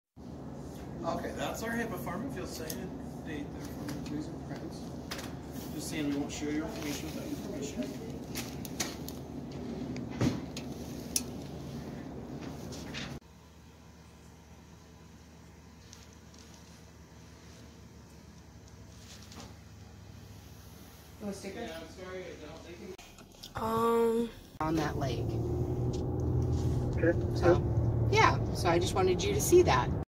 Had an injury yesterday at a football game!
I am wearing a brace and in crutches until the ortho says its fine